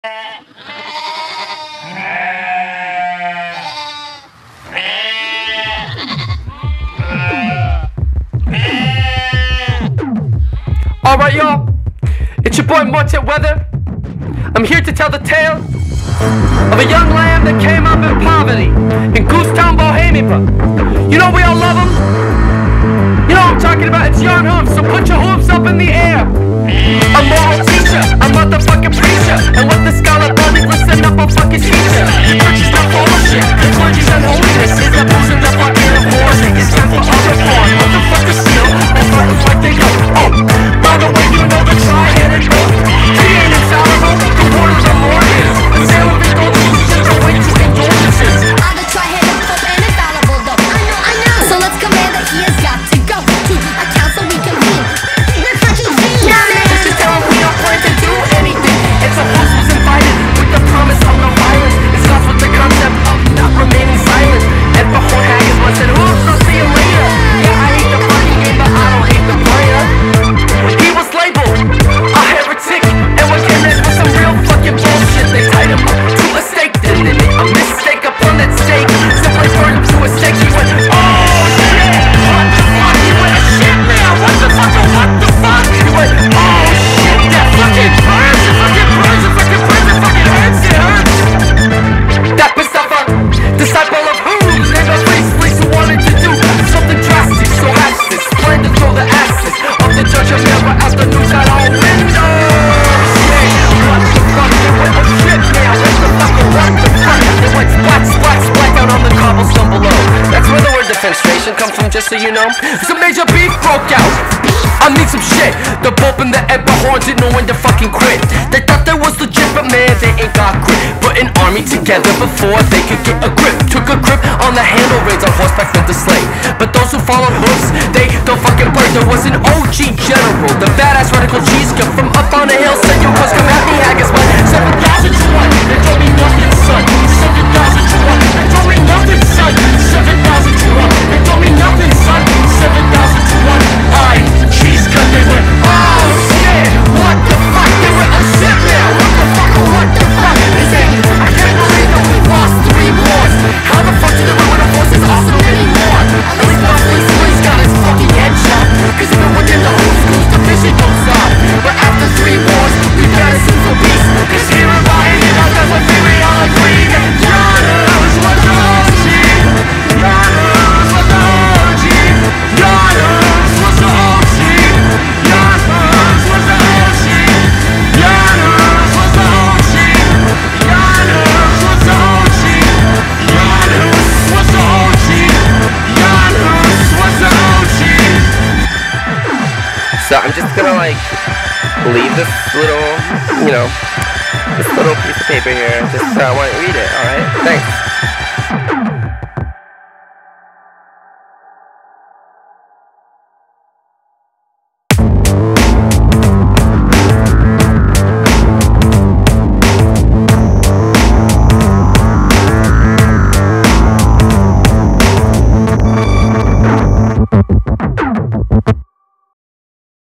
How Jan Hus Started a Reformation (Animated with Sheep)
An entry into the Sheepleverse based on the life of the Czech reformer - Martin Luther before Martin Luther - the one and only Mr. Jan Hus. If you are Czech, you will know this story from childhood, and if you have no idea about it your good old friend Wikipedia will help you out here:

and here is a description of the Andagainist Insurrection

This is sort of my ode to the Czech Republic and the city of Prague which I love dearly. While working on the Andagainist Insurrection there was this bit of history in the background that was informing it as well.

If and when I decide to dip my feet back into the Sheepleverse, the next entry will likely be about the Spanish Da Vinci Miguel Serveto. But that's another tail for another day

Music and Video by Mitch Marinello